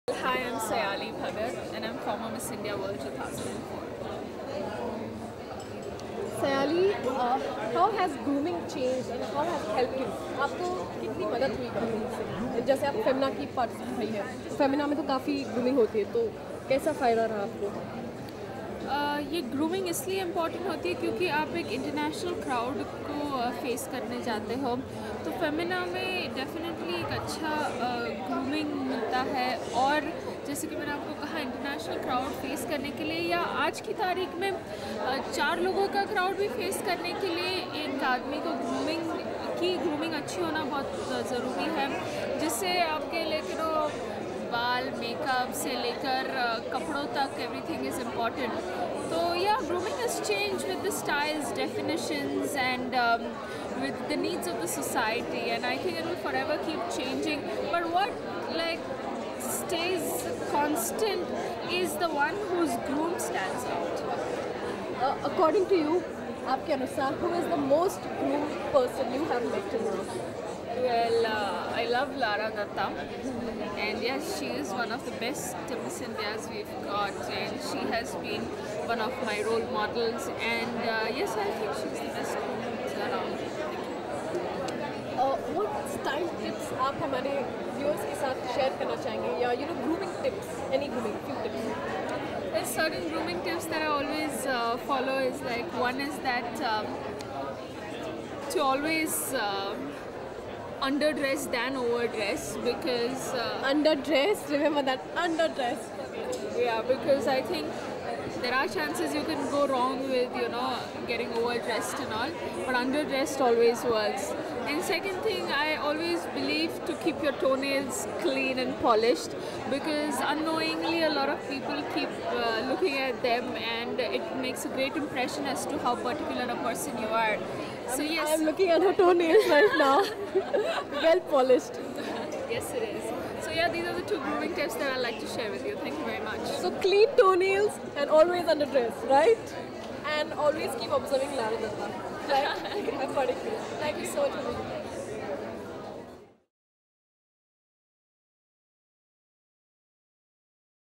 Sayali Bhagat, Former Miss India World, at Femina Miss India 2004: "I love Lara Dutta"
"This grooming is important as you're facing an international crowd. Even to face a four person crows, one needs to have the preparation from the hair and makeup to clothes. Grooming has changed with the needs of society.

I love Lara Dutta. She's one of the best Miss India's.